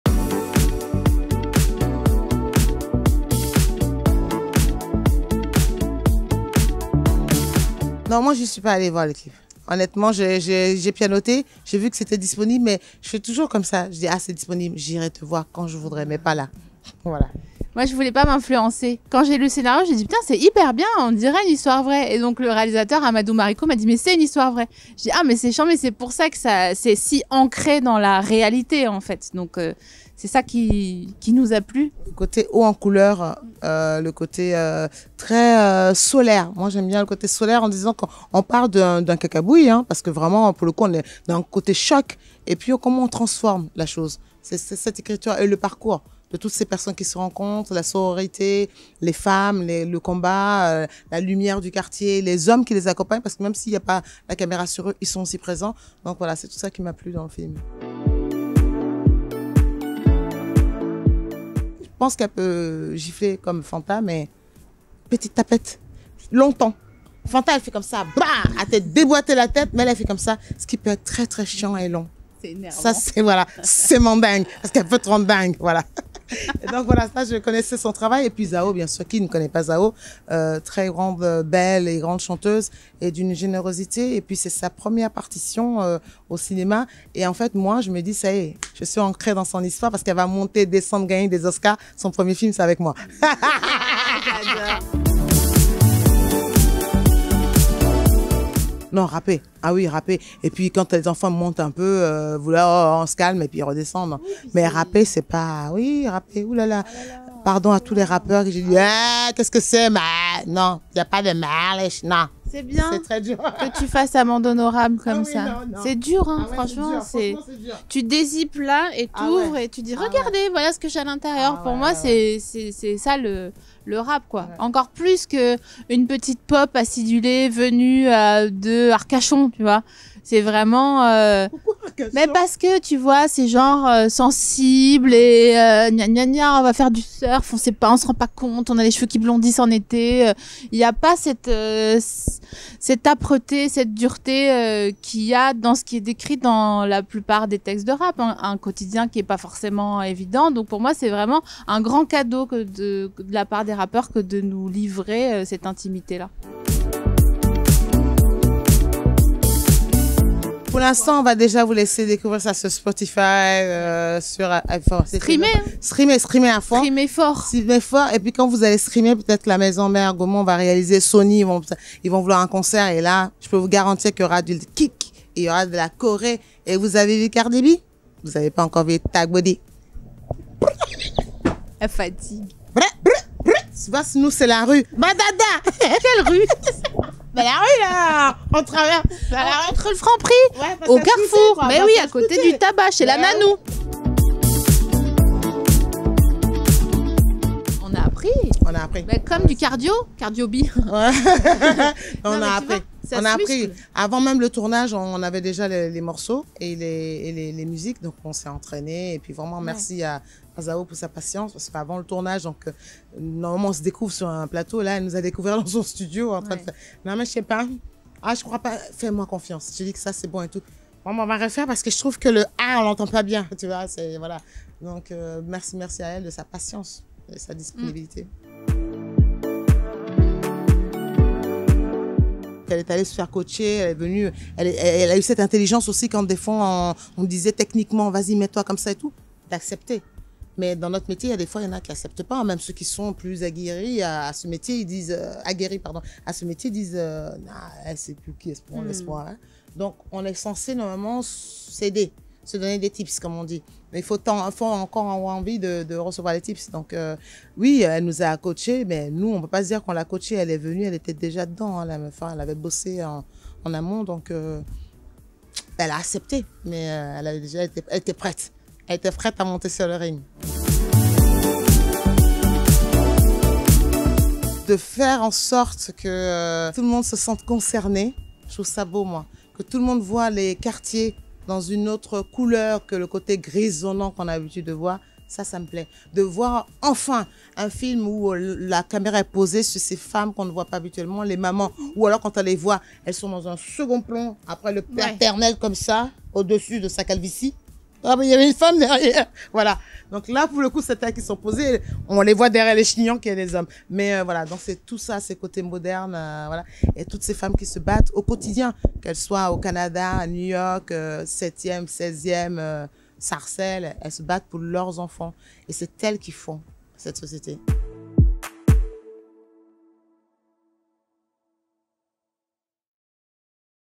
Comment Claudia Tagbo et Sophie-Marie Larrouy sont devenues des rappeuses pour Yo Mama ?
Dans la comédie Yo Mama sortie le 5 juillet dernier, Claudia Tagbo et Sophie-Marie Larrouy jouent des mamans qui, pour rétablir le dialogue avec leurs garçons de 11 ans, décident d’écrire un rap. Une manière aussi de répondre à leur clip polémique trop violent et vulgaire pour leur âge. Les deux actrices se sont confiées à Télé 7 Jours sur leur collaboration avec Zaho, leurs difficultés pour rapper et ce qu’elles aiment le plus dans cette comédie de Leïla Sy et Amadou Mariko.